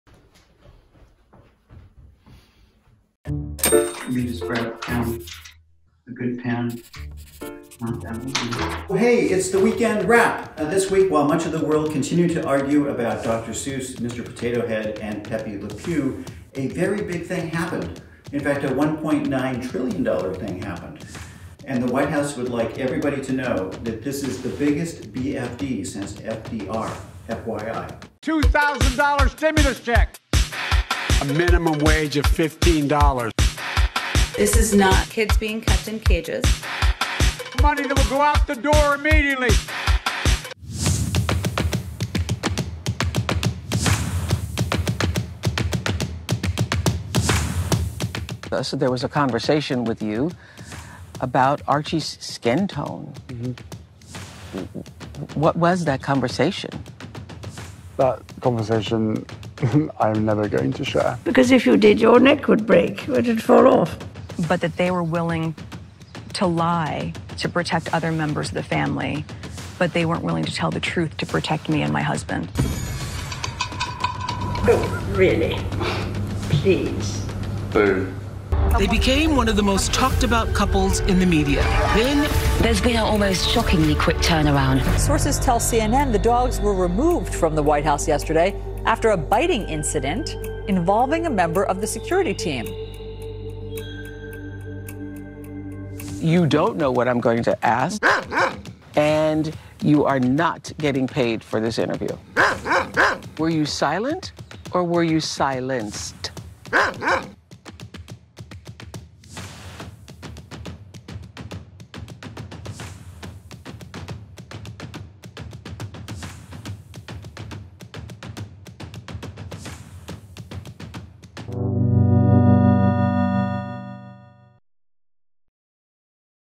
The political and royal tea of the week
Matt Wuerker takes you through the week in political satire and cartoons in the latest Weekend Wrap, including the Covid relief bill, the fallout from Meghan and Harry’s interview and the Dr. Seuss controversy.